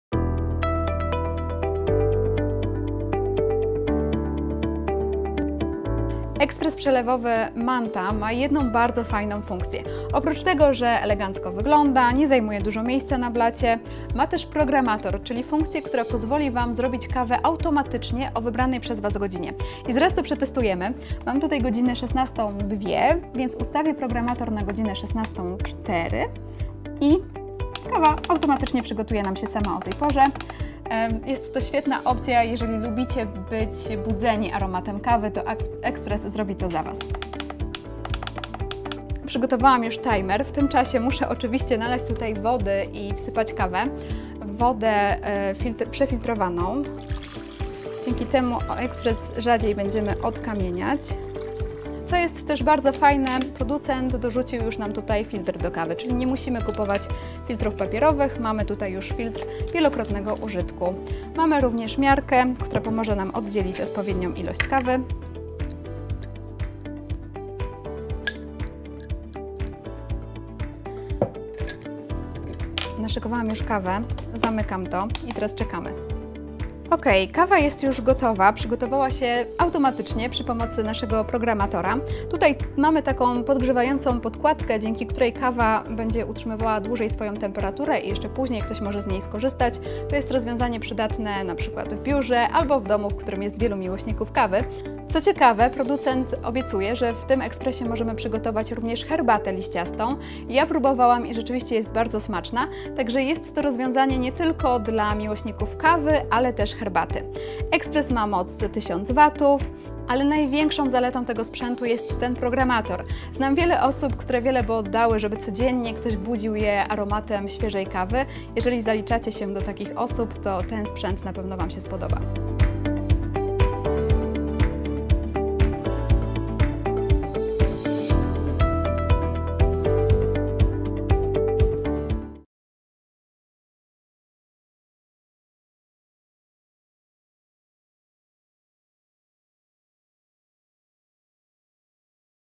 Carrefour testuje: EKSPRESY DO KAWY – Manta CMA 150T
Manta CMA 150T to zgrabny ekspres, który nie zajmuje dużo miejsca, a jego dużą zaletą jest programator – przygotuje kawę na wybraną godzinę. Obejrzyj nasz test i bądź z nami za dwa dni – tym razem przetestujemy ekspres kolbowy!